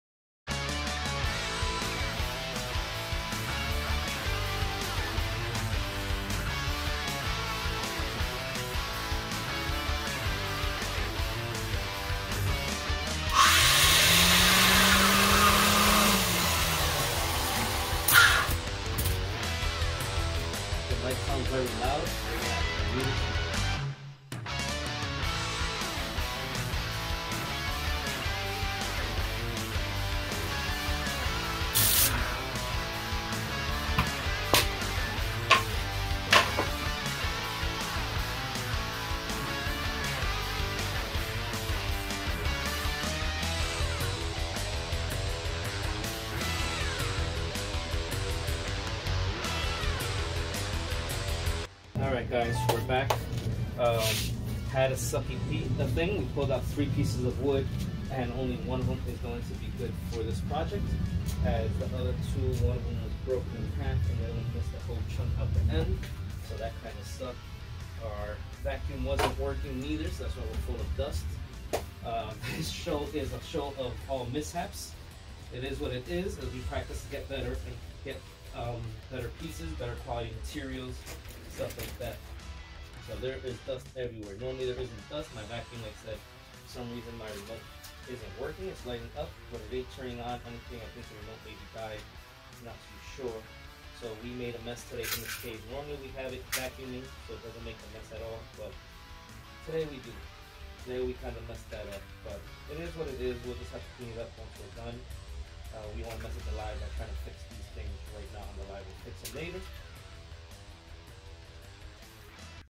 Sanding Drama: Crafting Brad Slagle's Prize for Green Nexus 1 Year anniversary of their Launch!!!
Join us for a dramatic twist in the crafting journey of Brad Slagle's prize from our one-year anniversary celebration at Green Nexus! Today, we're in the sanding process, but things took an unexpected turn. Out of the three pieces of wood we carefully selected, one was split down the middle, leading to a dramatic chucking with a knee chop. Another wood had a side splinter that tore out, leaving us with only one piece of wood to work with. Despite the challenges, we're determined to create a masterpiece for Brad. Don't miss the excitement unfolding in the next installment of our crafting series! 🌿✨